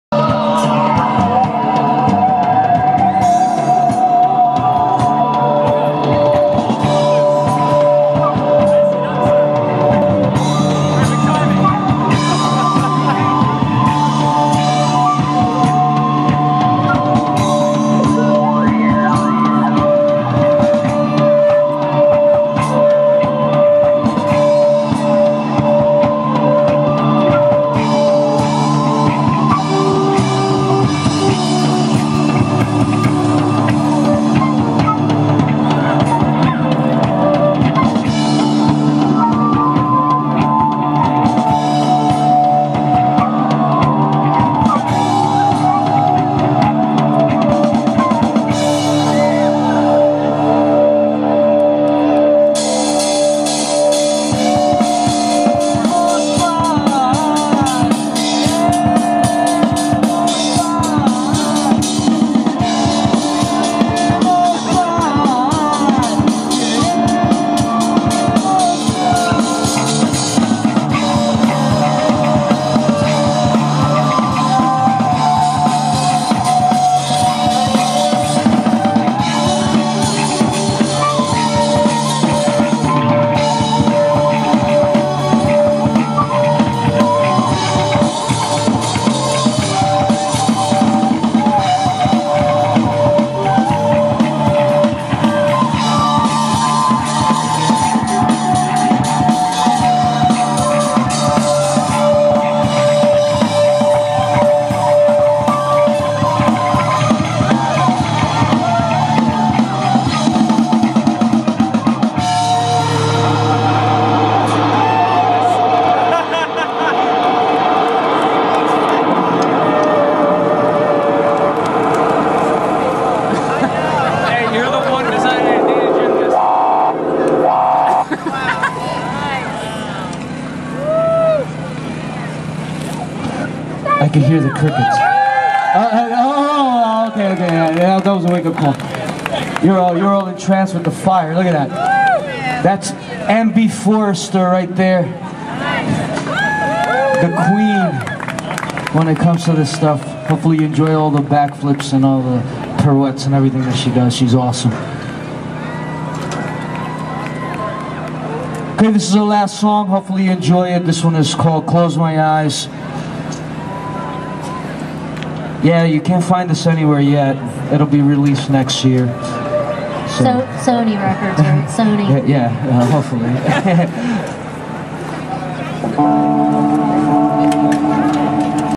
Phoenixville 2011 SnapDragon Fire Bracers
SnapDragon performing with her Fire Bracers at Phoenixville Firebird Festival 2011.
Live music performance by Stellarscope.